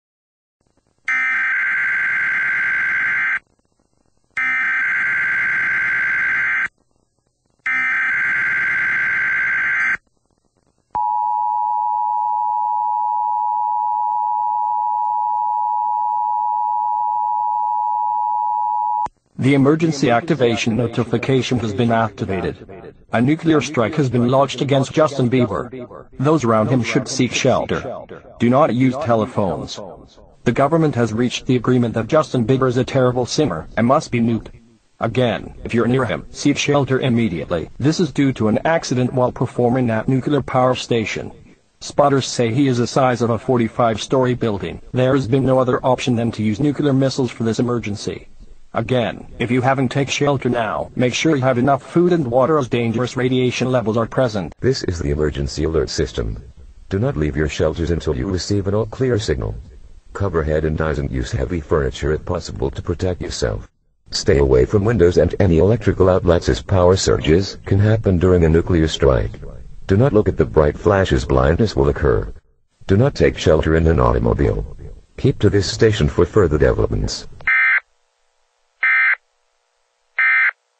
EAS: Justin Bieber Mutant Warning
Run!  It's a giant Justin Bieber!!  An EAS mock-up of what would happen if Justin Bieber was a radioactive mutant and had to be destroyed by a nuclear attack.